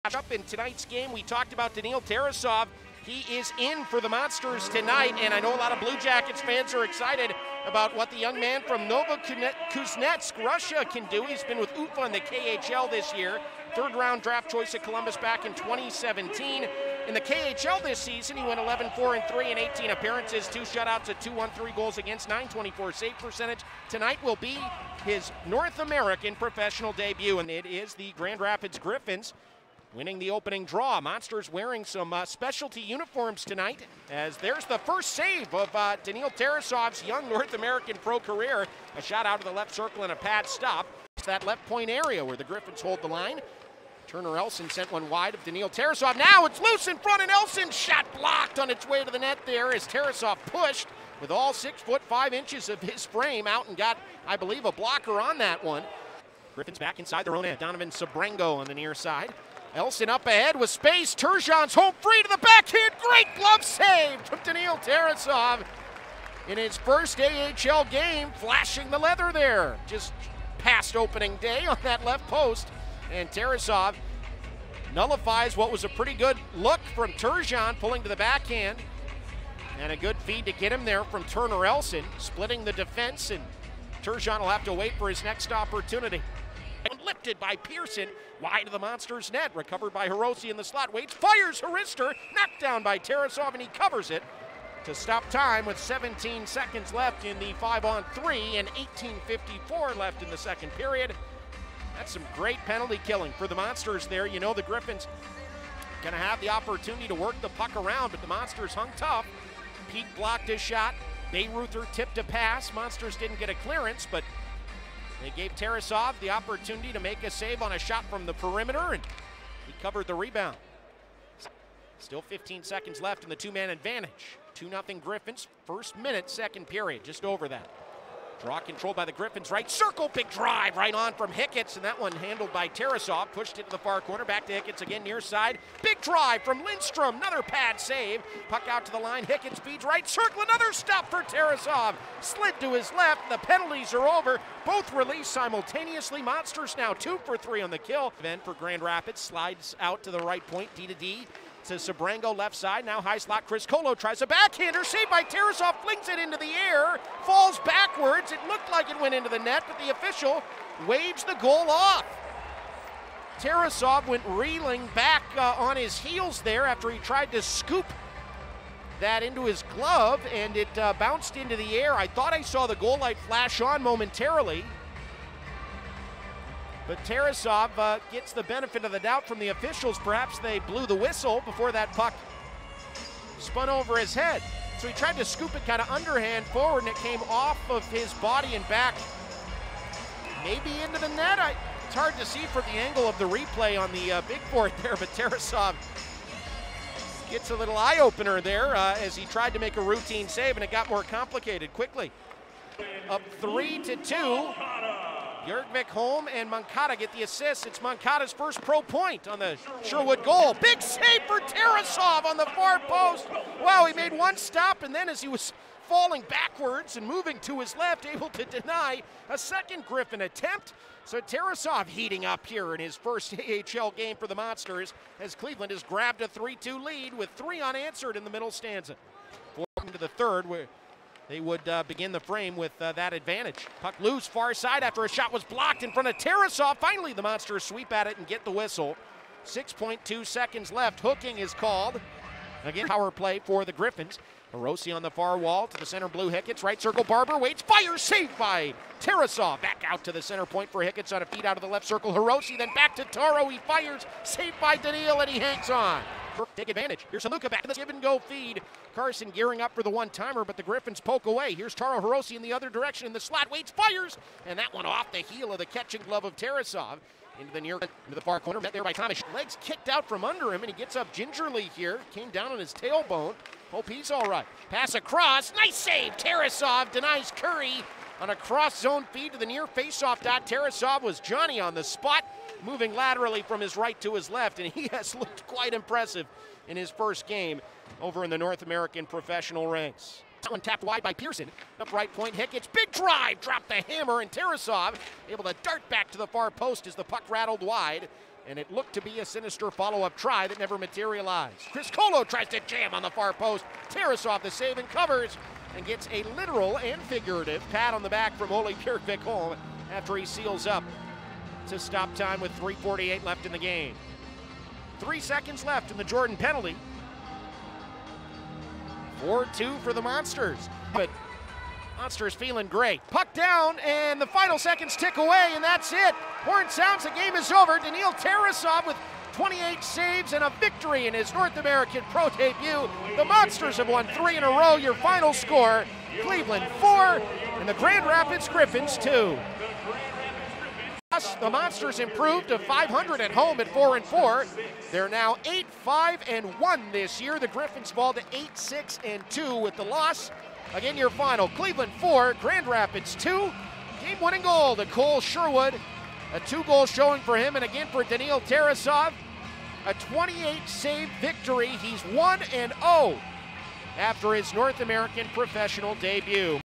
Full Highlights of Daniil Tarasov's AHL Debut (Apr. 3, 2021)
Daniil Tarasov was very impressive in his North American pro debut, stopping 27 of 29 shots, including a couple beauties, en route to his first career AHL win. Tarasov was also named the 1st star of the game.